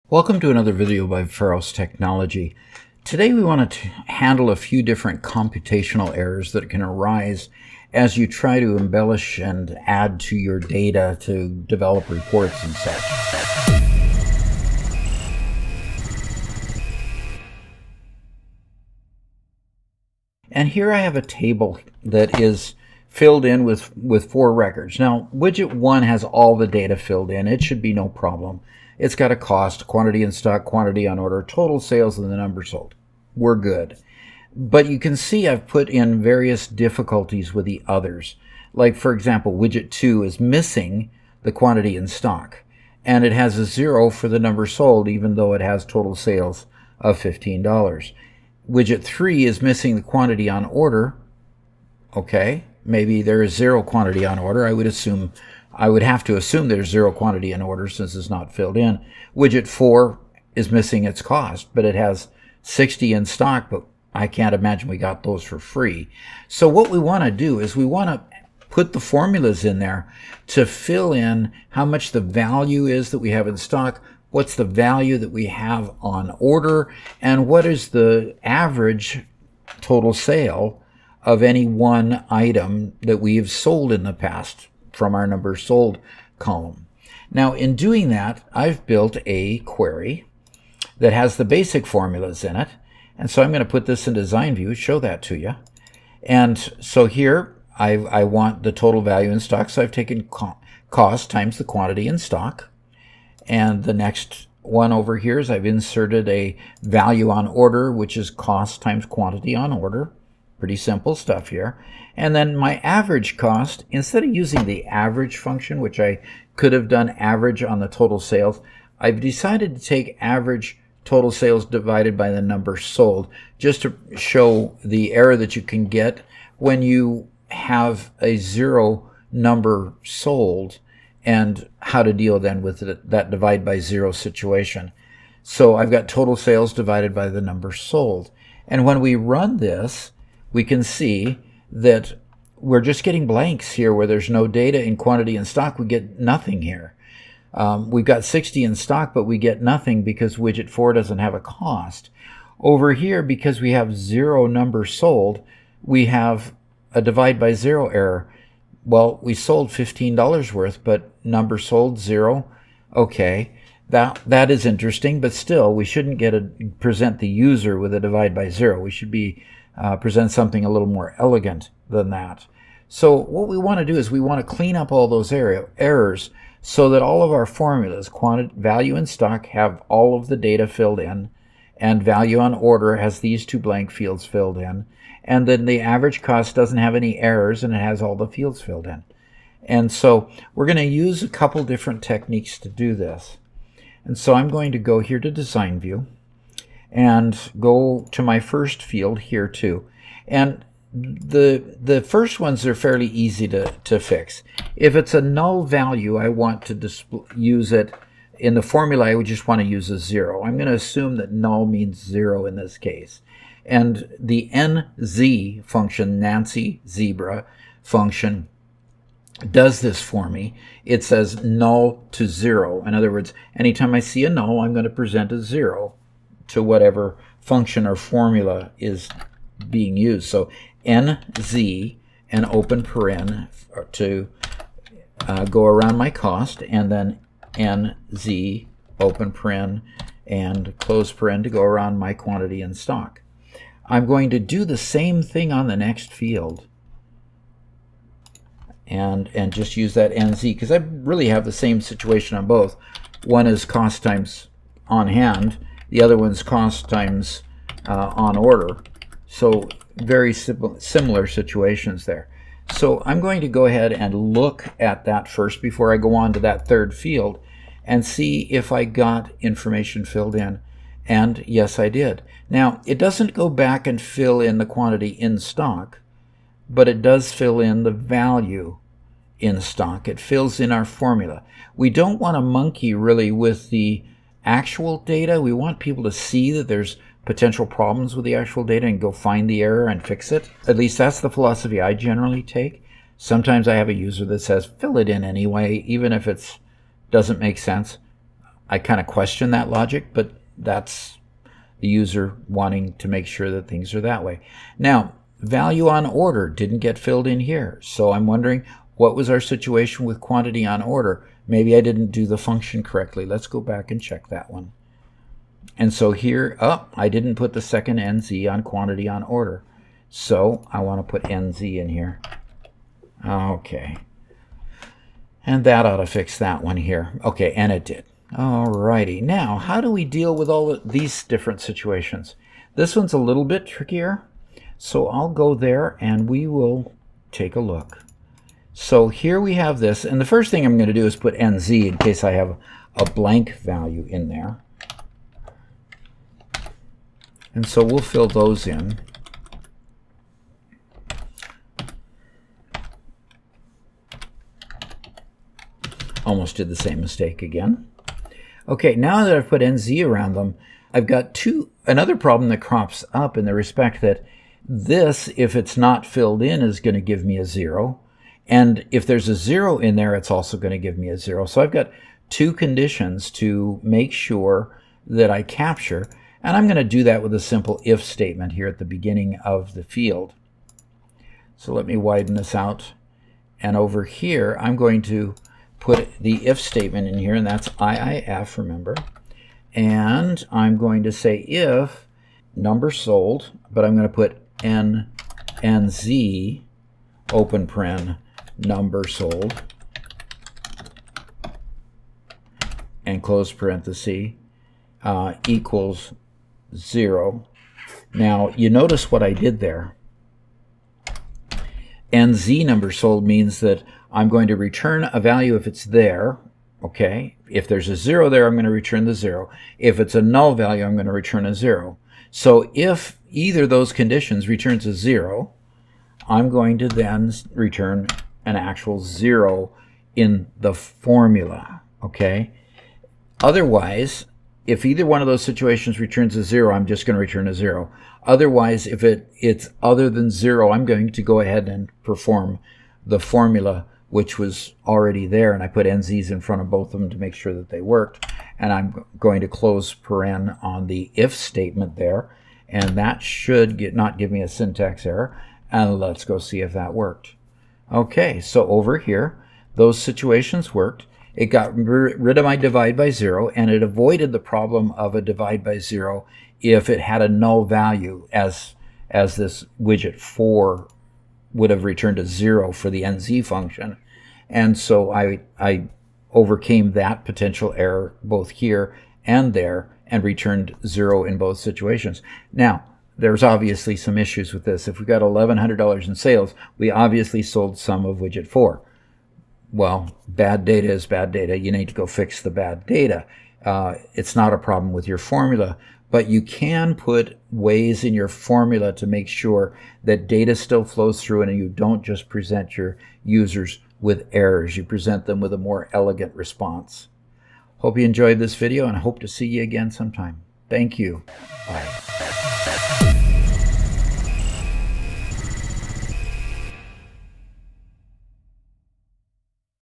Handling Computational Errors and Null Values in Access Queries
As programmers, we need to assume that the data is bad and need to code for those anomalies. Handling values that are not filled in (Nulls), values that are blank, and values that are unexpectedly 0 when we want to divide by that number can create ugly errors that the user will see in reports. In this video, we address common computational errors that may occur when working with data in a table, especially when dealing with missing or incorrect values. Learn how to use various techniques and functions, such as the NZ and IIF functions, to handle these errors and present more elegant results in your queries.